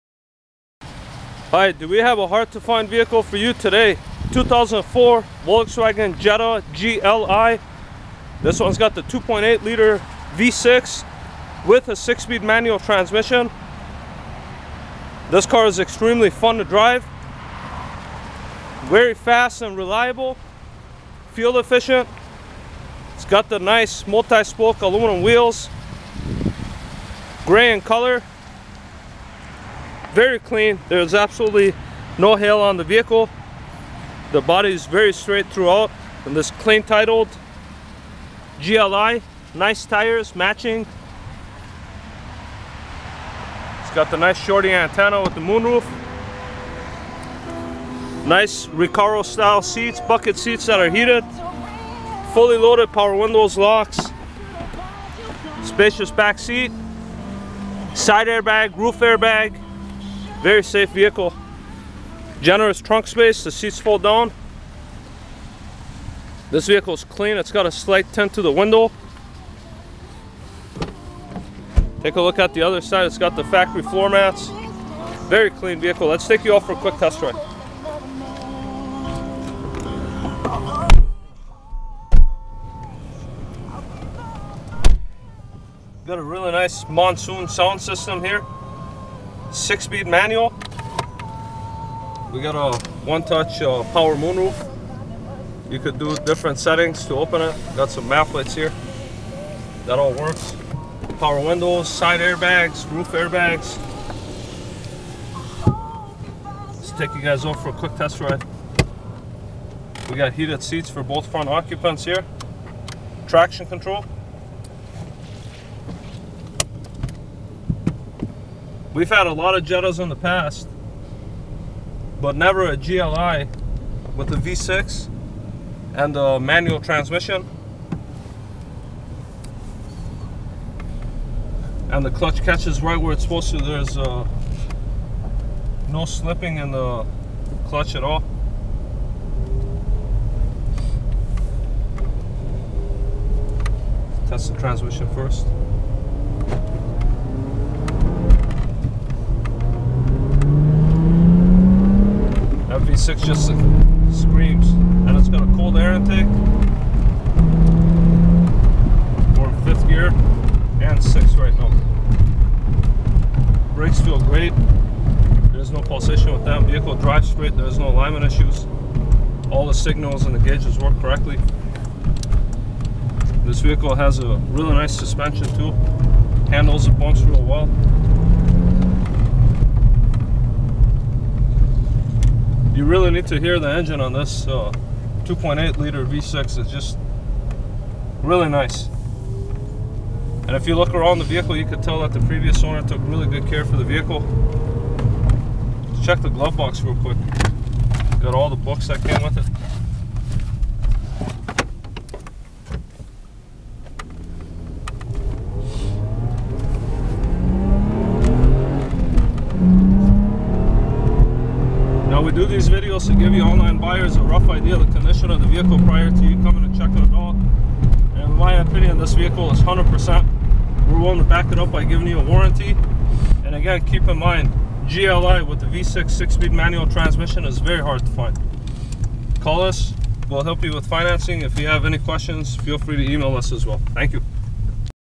2004 VOLKSWAGEN Jetta GLi, 2.8 VR6, 6 speed, NICE !!!
4 door sedan, 2.8 liter VR6, 6 speed manual transmission, Air conditioning, fully loaded with power options, CD player, fully loaded, power moon roof, heated seats, 17" inch alloy wheels, VERY SHARP AND CLEAN !!!